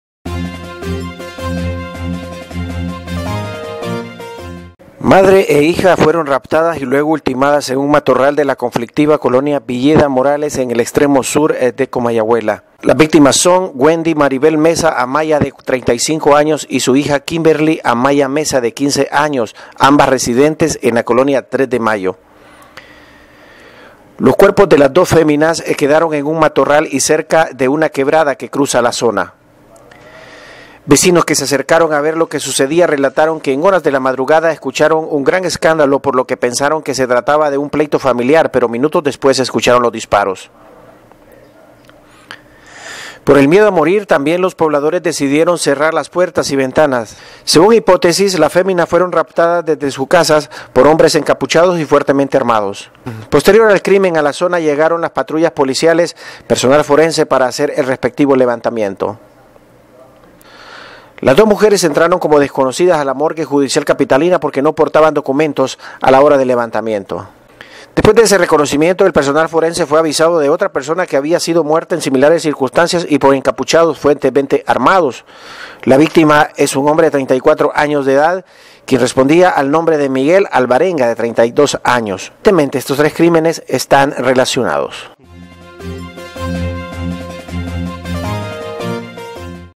Diario La Tribuna (Honduras)
Raptan y matan a madre e hija en la Villeda Morales